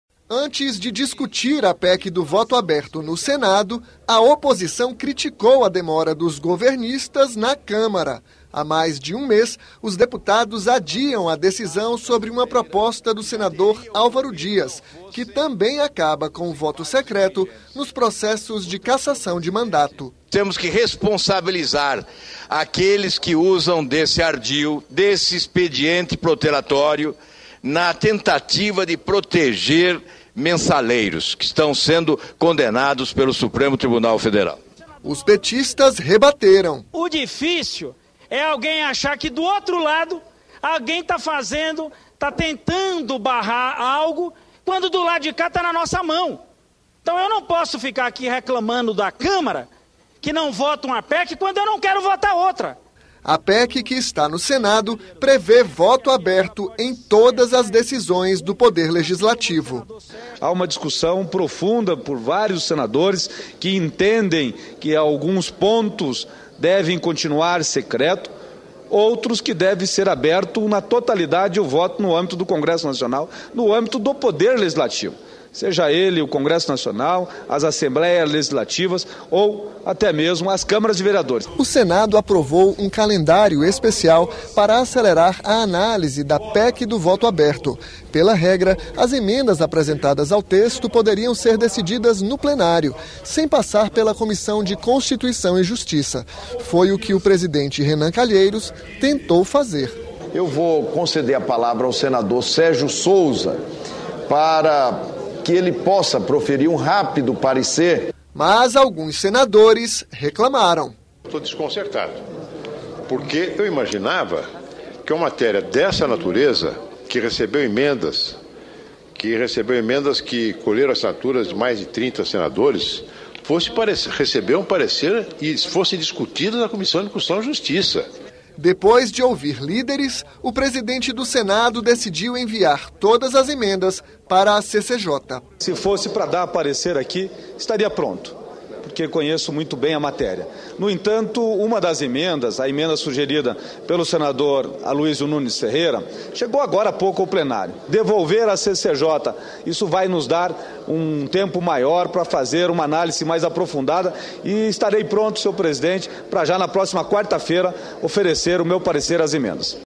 Veja como foi a discussão em Plenário sobre a PEC do voto aberto
A PEC 43/2013, que prevê o voto aberto nas votações em todas as esferas do Legislativo no país, teve nesta quarta-feira (2) a quinta e última sessão de discussão do primeiro turno de votação no Plenário. Por ter recebido emendas, a proposta voltou para a Comissão de Constituição e Justiça.